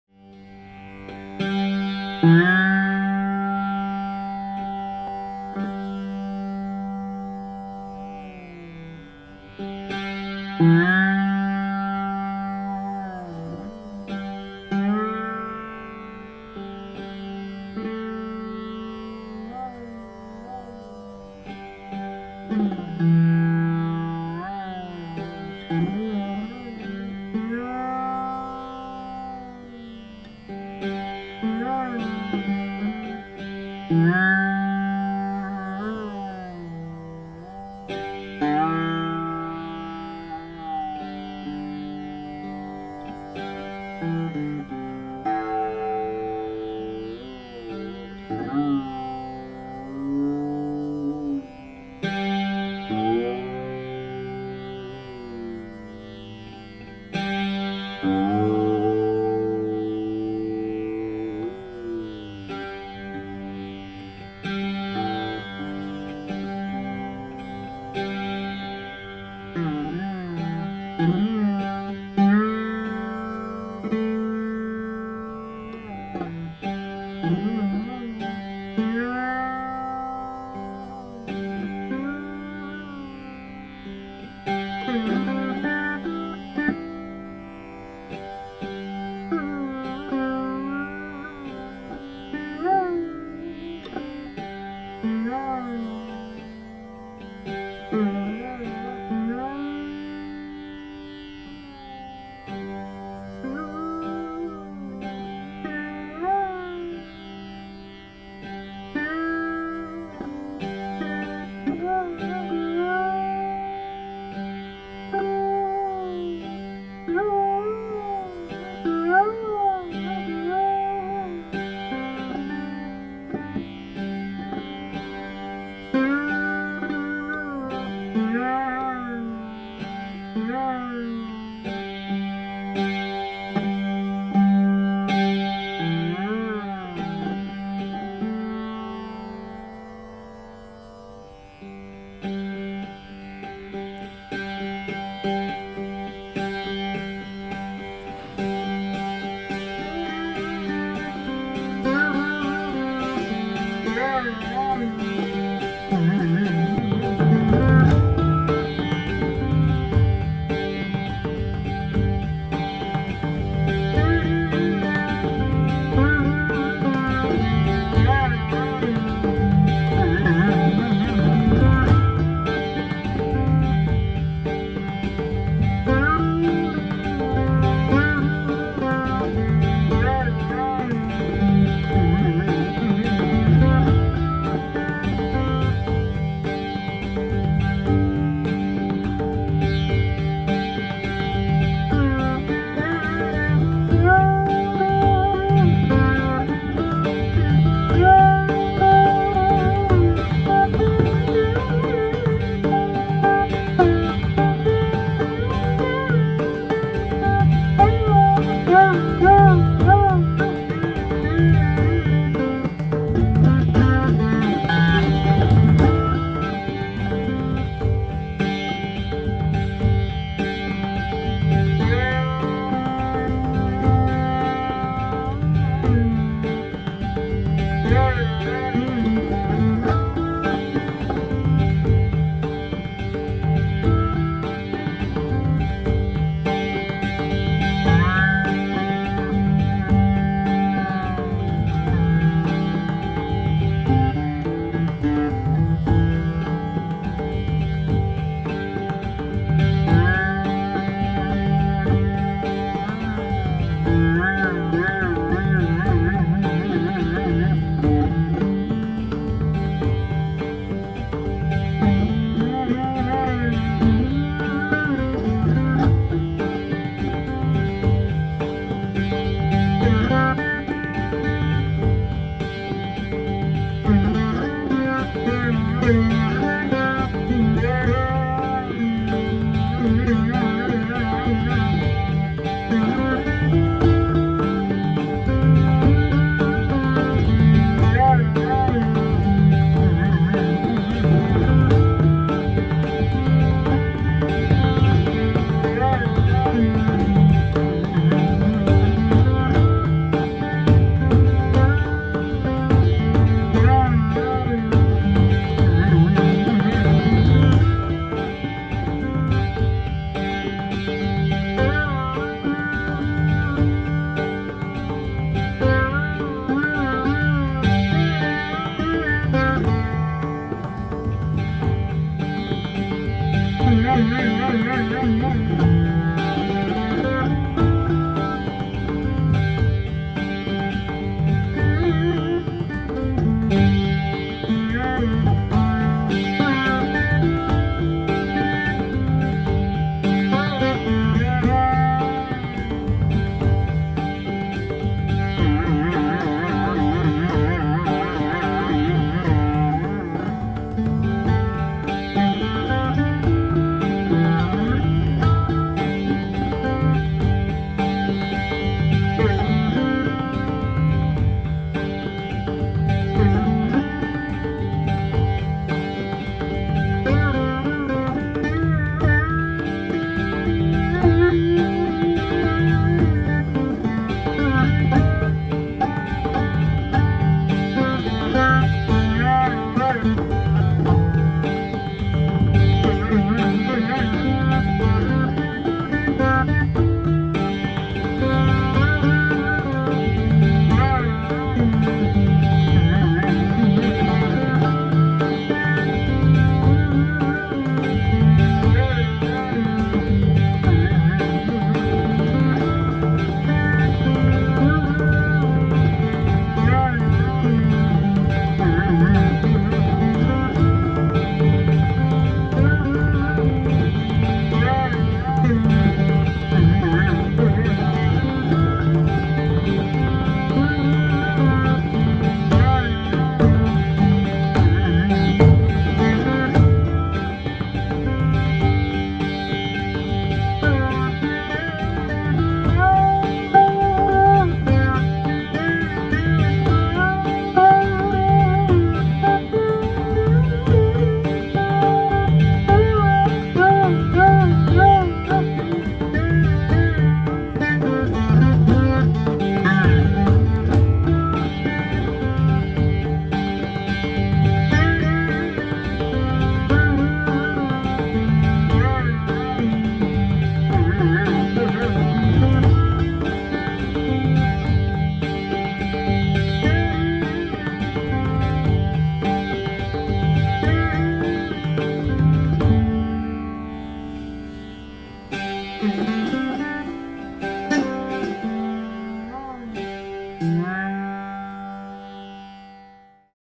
Raag Durga | Dhrupad in Teevra Taal | Rudra Veena & Pakhawaj
This composition was originally composed by my Guruma Vidushi Mita Nag as a greeting for the famous Bengali Durga Pooja back in 2015/16. It stayed with me throughout all these years and then took this shape on my Veena. I have done some modifications for the composition to suit Dhrupad Instruments but I can never claim credit for this composition.

Raag Durga - Short Alaap Followed by a Composition in Teevra Taal.

Rudra Veena : Madhuvanti Pal
Pakhawaj : Mithun Chakraborty

We are also celebrating Barbara’s birthday today. Check out her channel @Dhrupad Love and Cats